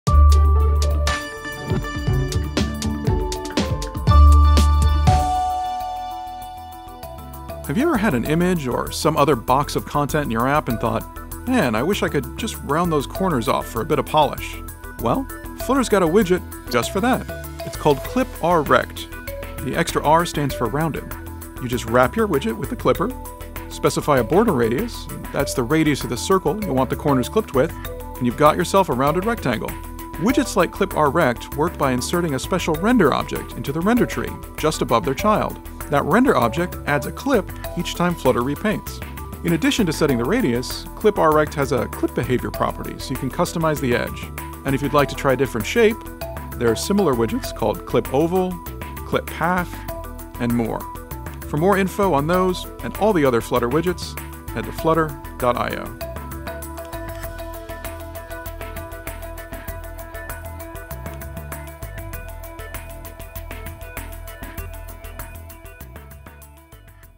ClipRRect (Flutter Widget of the Week)
For all those times you’ve wished you could round the corners on a box of content in your app, there’s ClipRRect (the extra R actually stands for rounded)! Wrap your widget with the clipper, set the radius, and it does the rest! You can even customize the look and try different shapes with similar widgets. Give it a go!

This video is also subtitled in Chinese, Indonesian, French, German, Japanese, Korean, Portuguese, and Spanish.